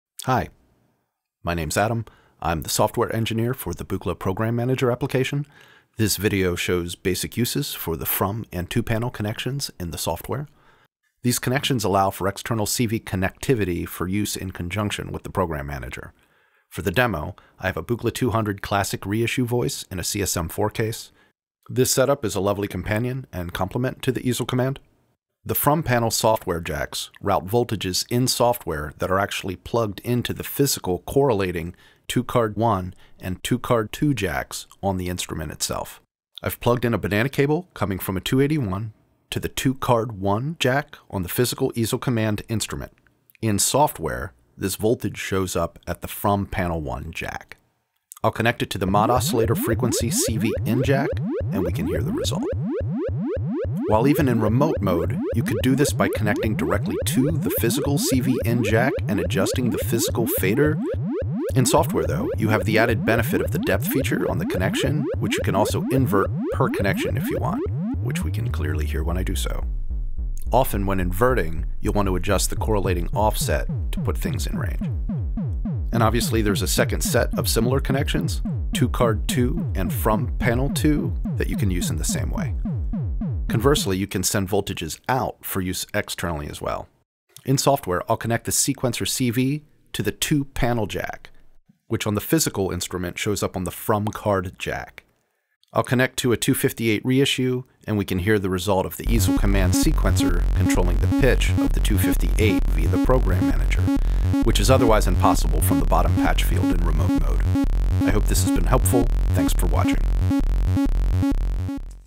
Buchla Program Manager: To/From Card & Panel Connections
This video shows basic uses for the FROM & TO PANEL connections in the Program Manager software.

We’ve received questions regarding these and hopefully this will help clarify their use and benefit. This demo is intended to show functionality simplistically vs creating something intended to sound musical

These connections allow for external CV connectivity for use in conjunction with the PROGRAM MANAGER.